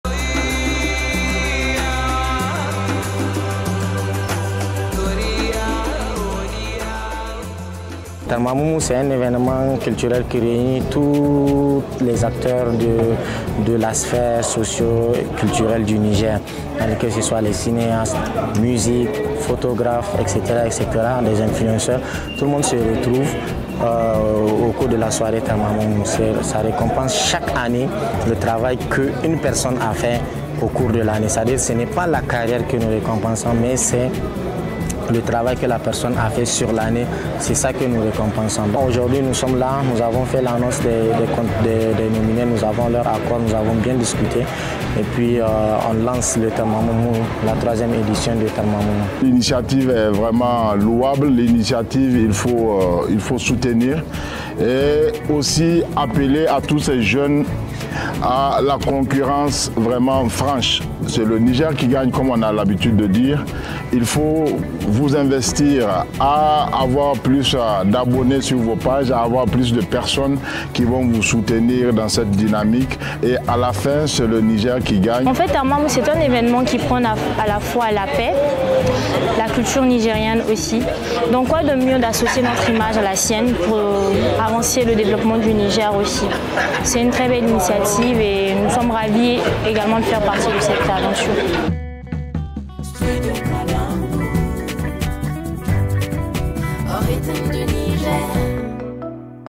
Conférence de presse Tarmamun mu 3éme édition
Le comité d’organisation de la Cérémonie des Awards du Niger Tarmamun Mu a organisé une conférence de presse le samedi 02 Mars à Niamey.
C’est pour annoncer les nominés de la 3ème édition